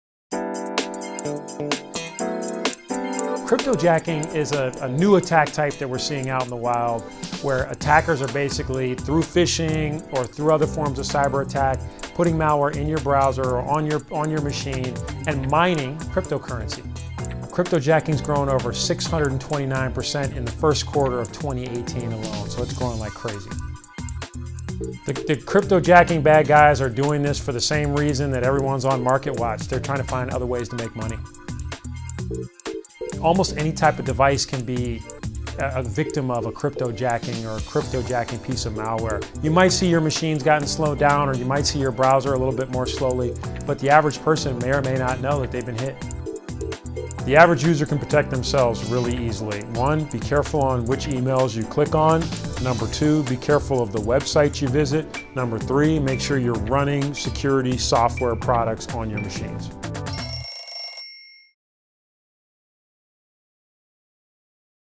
On The Clock: What is cryptojacking?
In this episode of 'On The Clock', McAfee CEO Chris Young breaks down 'cryptojacking', a new type of cyberattack, and explains how you can protect your devices.